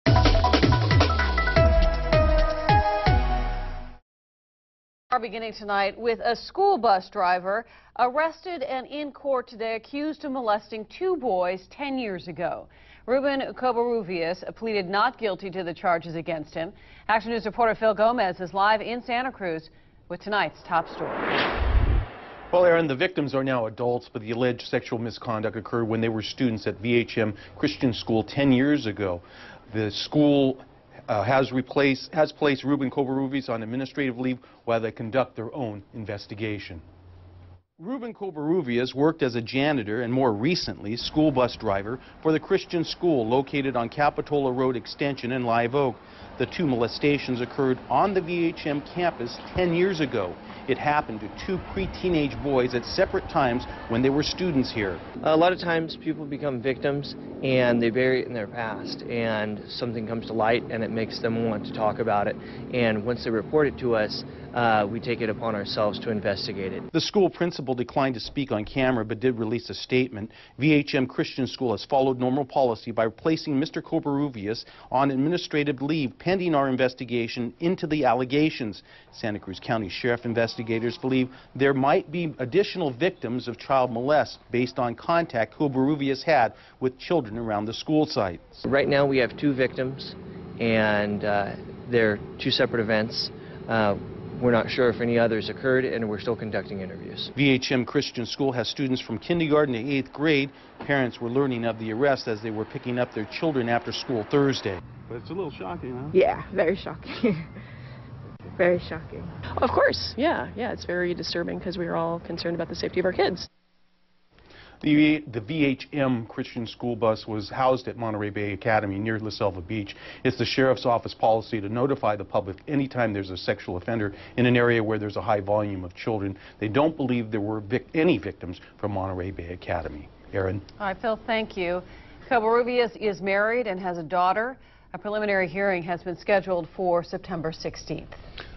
A school bus driver arrested
A Live Oak Christian school bus driver is under arrest, accused of molesting kids.